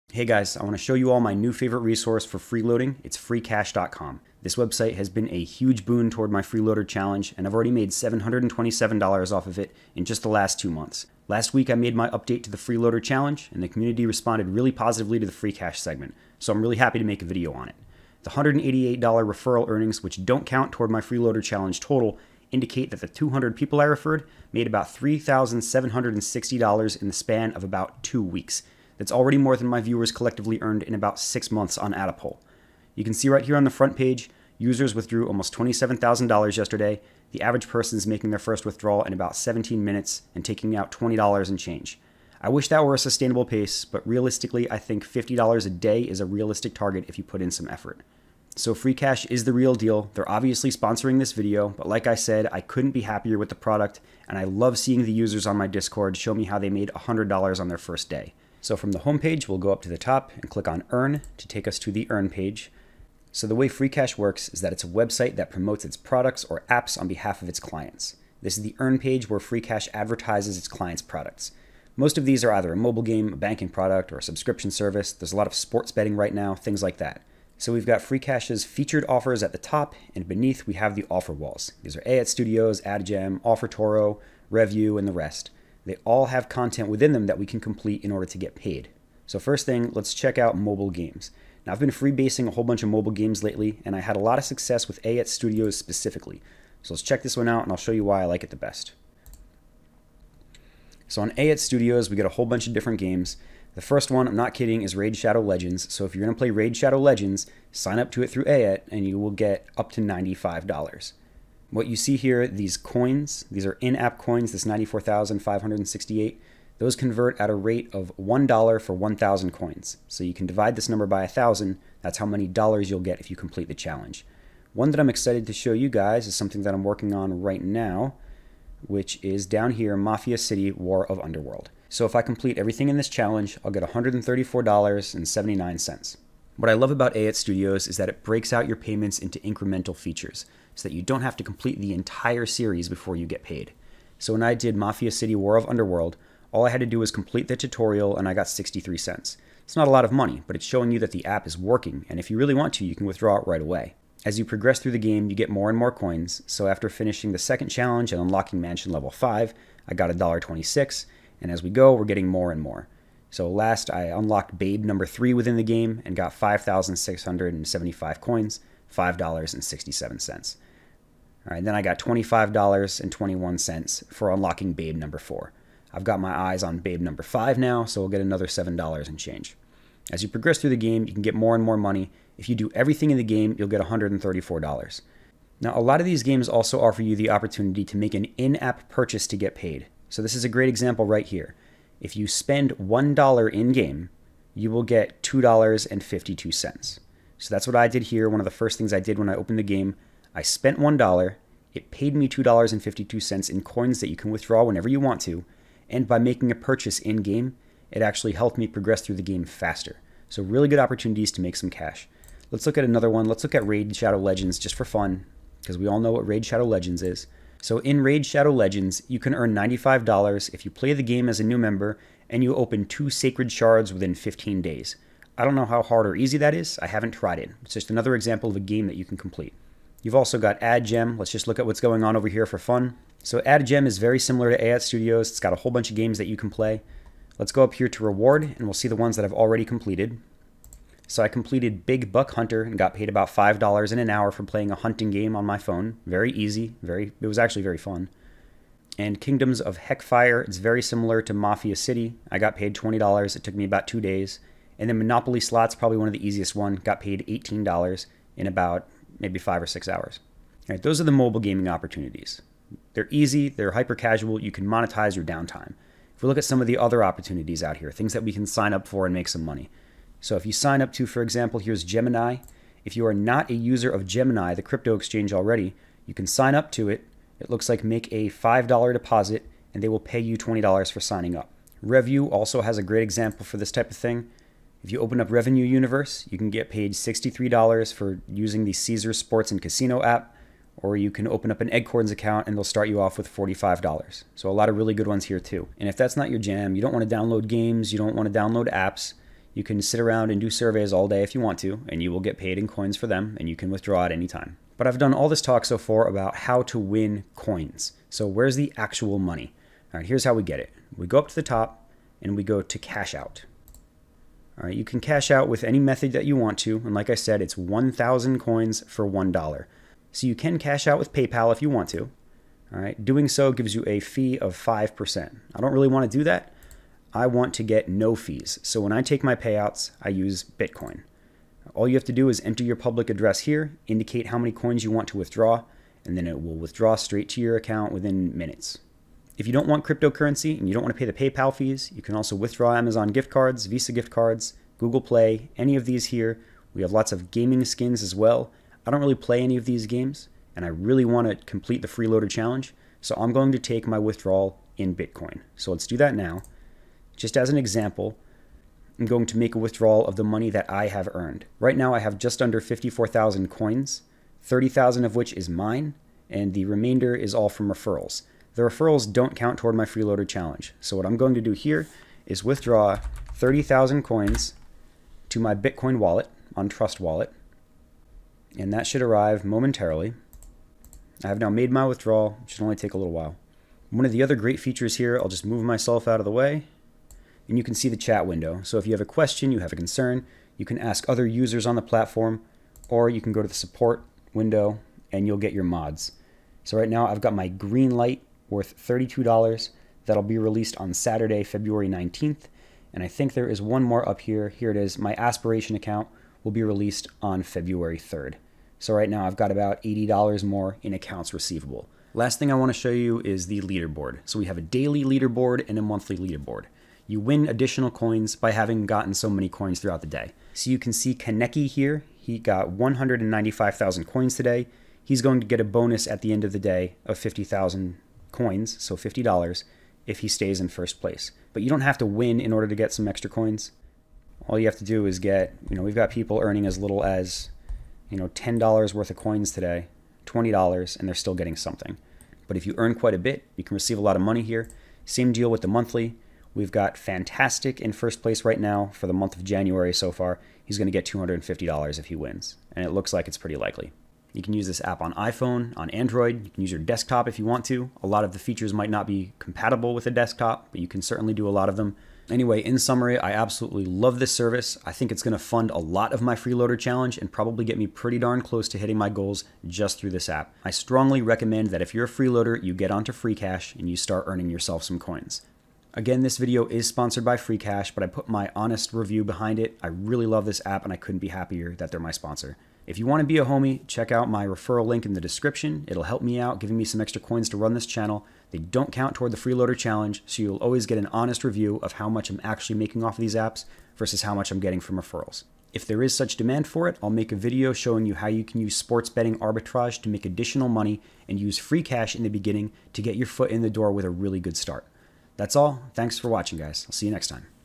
The Best Website to Make Money Online (For Students)
Freecash was created for students who want to make money online, by completing tasks on their phone. Get paid for testing websites, apps or completing surveys.